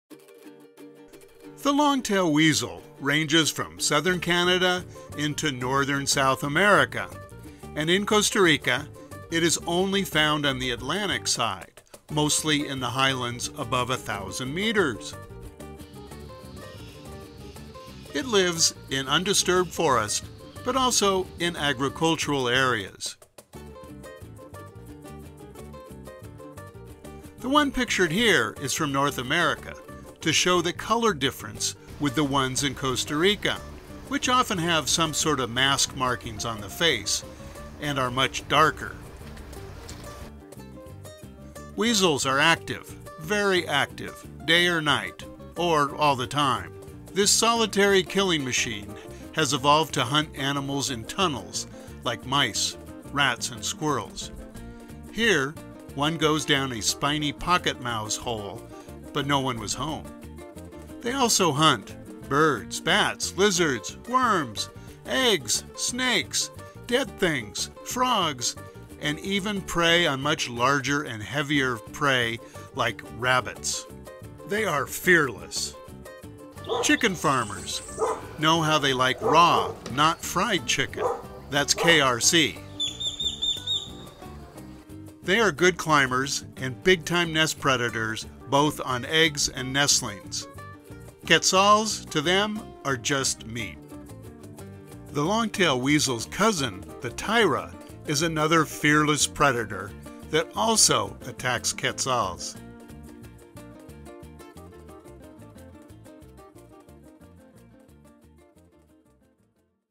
The Long-Tailed Weasel- A Hyper Active Mouse Killer
The Long-Tailed Weasel lives mostly in the highlands of Costa Rica but it is less common in the Caribbean lowlands.